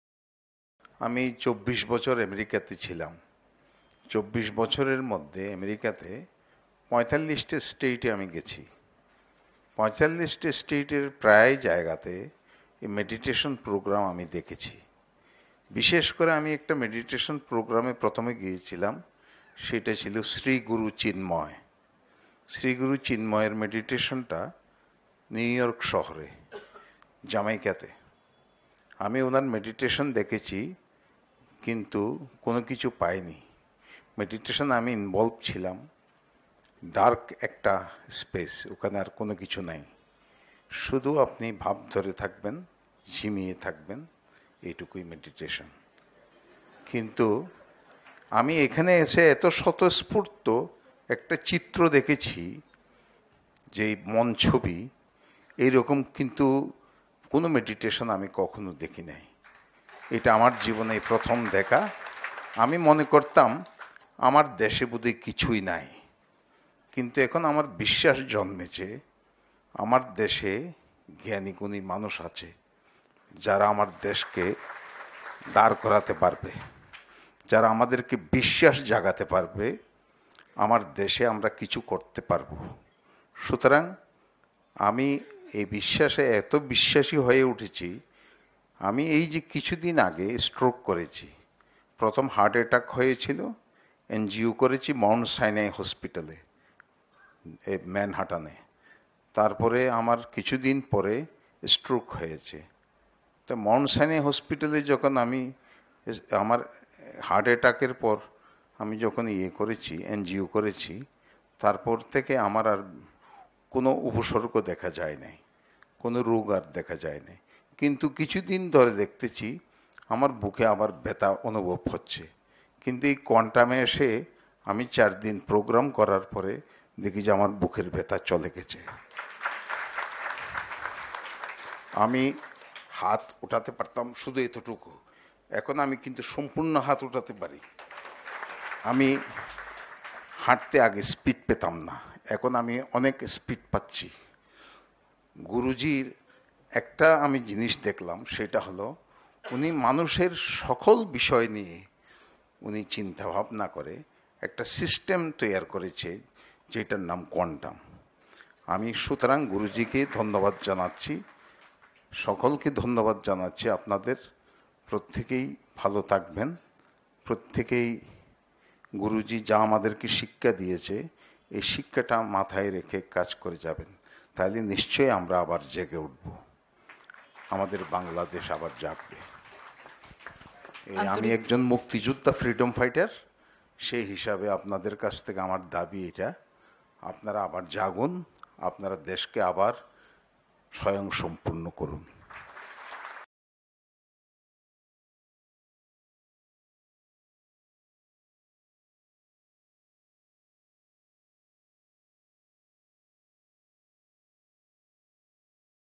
আমার বুকের ব্যথা চলে গেছে || My Chest Pain Disappeared after Coming Here
Abdul Kader Chowdhury is a Bangladeshi Painter. He had been living in the USA for over 24 years. He used to attend some other meditation system.

He has attended 374th Batch of Quantum Method Course.

In this video he has shared that the meditation course, autosuggestion and meditation practice has removed his chest pain. He says, 'I have found a spontaneous scene here!'